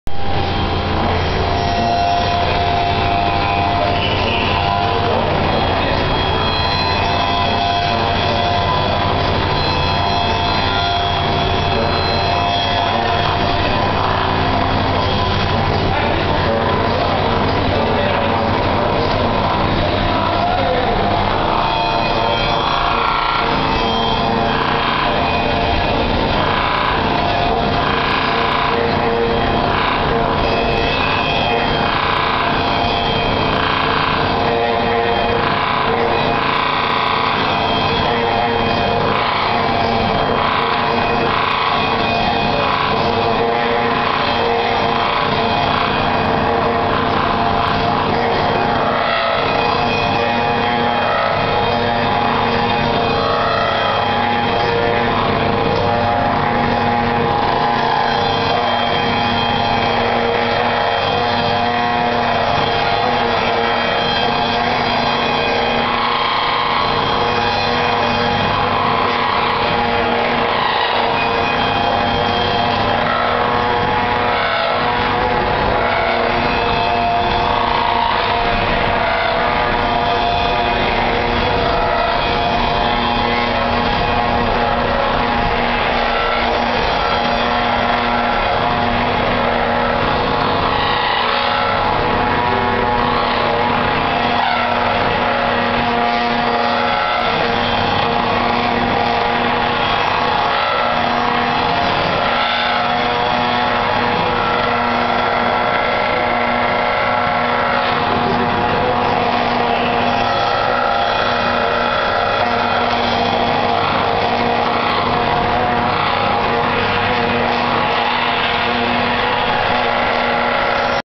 "Ankunft: Neue Musik" 2012 im Berliner Hauptbahnhof - eigenwillige Klangperformance
Klingt wie die Baustelle nebenan - die mobile Konzert-Performance um 22h vom Trio Heenan / Müller / Vorfeld